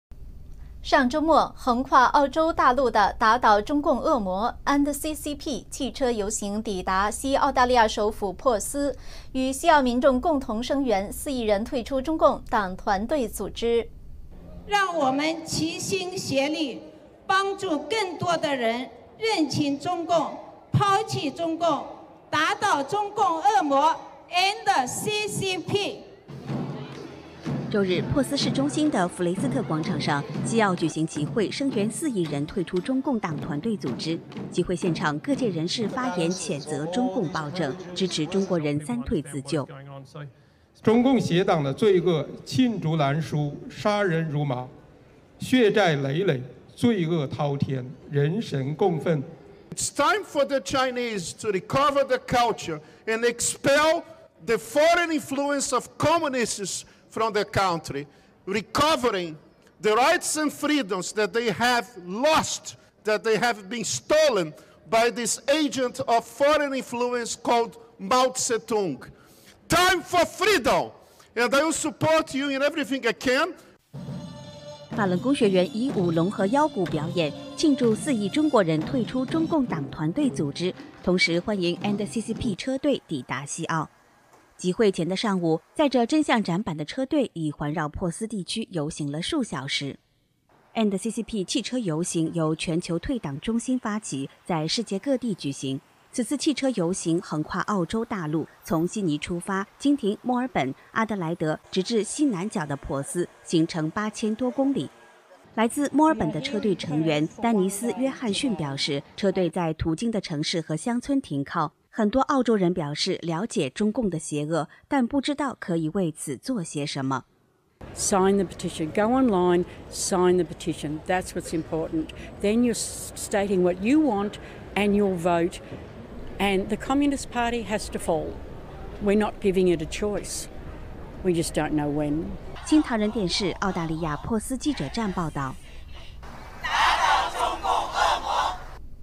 End CCP环澳车游抵珀斯 声援4亿人三退【三退】
【新唐人北京时间2022年08月24日讯】上周末，横跨澳洲大陆的End CCP汽车游行抵达西澳大利亚首府珀斯，与西澳民众共同声援，4亿人退出中共党、团、队组织。

End CCP车队协调人陈芳芳：「让我们齐心协力，帮助更多的人认清中共，抛弃中共，打倒中共恶魔，End CCP。」

周日珀斯市中心的弗雷斯特广场上，西澳举行集会声援4亿人退出中共党团队组织。集会现场，各界人士发言谴责中共暴政，支持中国人三退自救。

珀斯退党中心负责人盛正平：「中共邪党的罪恶罄竹难书，杀人如麻，血债累累，罪恶滔天，人神共愤。」

法学教授Augusto Zimmermann：「中国人应该恢复自己的传统文化，把外来的共产主义驱出国门，恢复应有的自由，多年来，这些自由被受到外来影响的毛主义窃取了。自由的时候到了，我尽我所能支持你们。」

法轮功学员以舞龙和腰鼓表演，庆祝4亿中国人退出中共中共党、团、队组织，同时欢迎End CCP车队抵达西澳。

集会前的上午，载著真相展板的车队已环绕珀斯地区游行了数小时。

End CCP汽车游行由全球退党中心发起，在世界各地举行。此次汽车游行横跨澳洲大陆，从悉尼出发，经停墨尔本、阿德莱德，直至西南角的珀斯，往返行程八千多公里。

来自墨尔本的车队成员丹妮斯·约翰逊表示，车队在途经的城市和乡村停靠，很多澳洲人表示了解中共的邪恶，但不知道可以为此做些什么。

End CCP车队成员Denice Johnson：「到（endccp.com）网站上签名，很重要，表达你们想要的，表达你们的支持。中共一定会倒台，它没有出路，仅仅是时间问题。」

新唐人电视台澳大利亚珀斯记者站报导